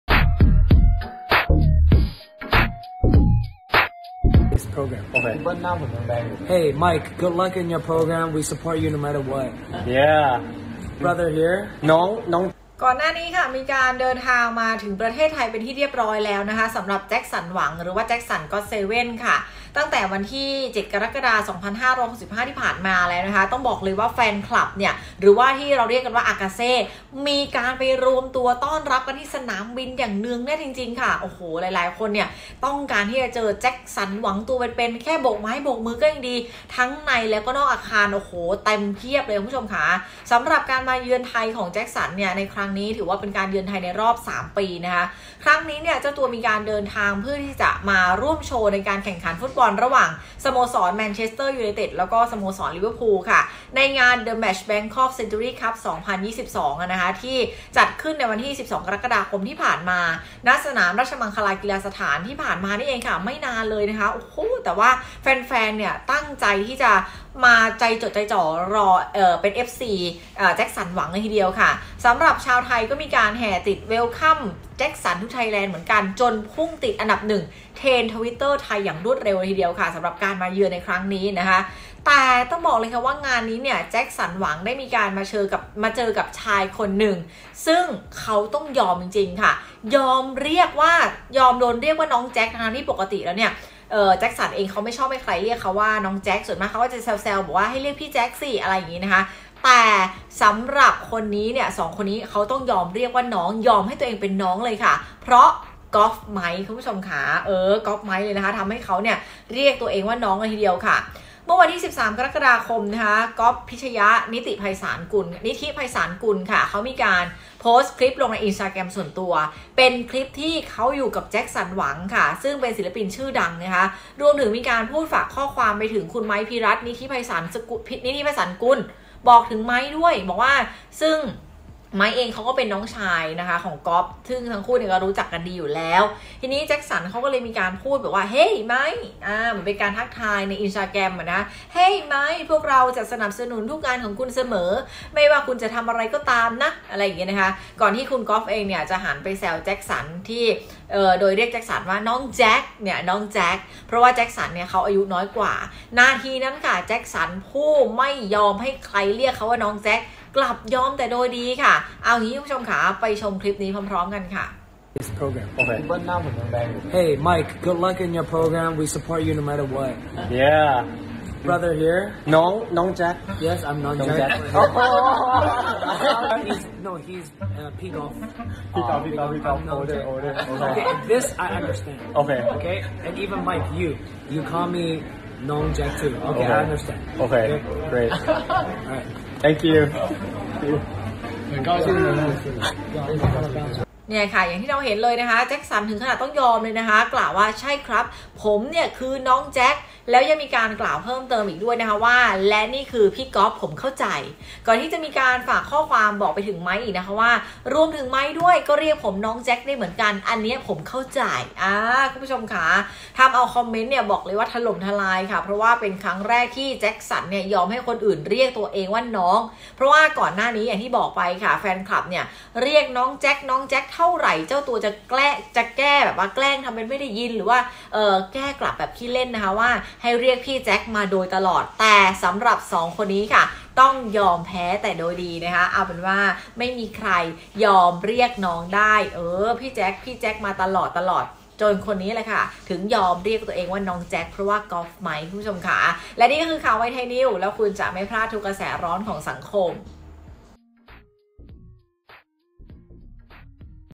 “แจ็คสัน หวัง” ยอมโดนเรียก “น้องแจ็ค” จากไม่เคยยอมใคร ต้องยอมให้สองคนนี้?| Thainews - ไทยนิวส์
Jackson Wang (แจ็คสัน หวัง) ต้องยอม! โมเมนต์น่ารักที่แจ็คสัน หลัง ที่ขอถูกเรียกว่า “พี่แจ็ค” เท่านั้น ยอมโดนเรียก “น้องแจ็ค” เพราะกอล์ฟ-ไมค์

jacksonwang,แจ็คสันหวัง,
แจ็คสันgot7,jacksongot7,
กอล์ฟพิชญะ,ไมค์พิรัชต์,
กอล์ฟ-ไมค์